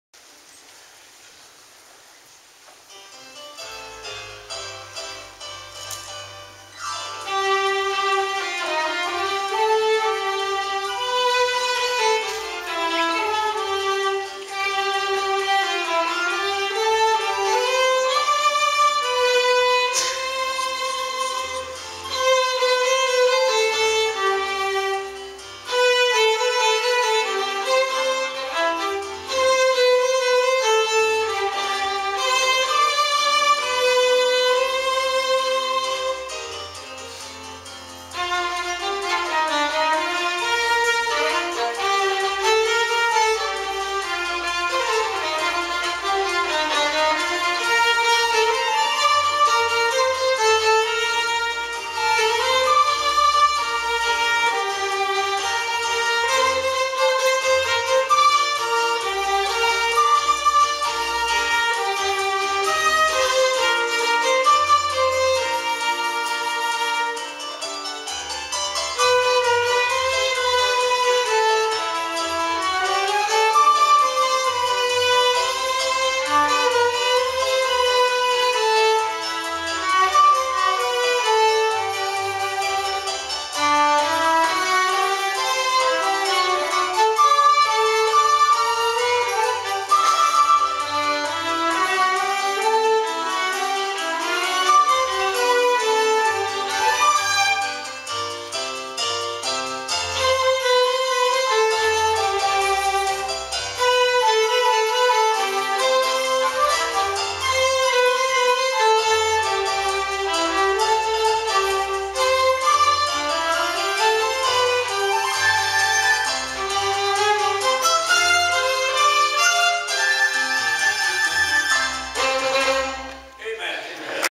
Heather Matthews Violin
A Medley of Songs for Wednesday Night Teen Camp
Festus, MO
6-24-15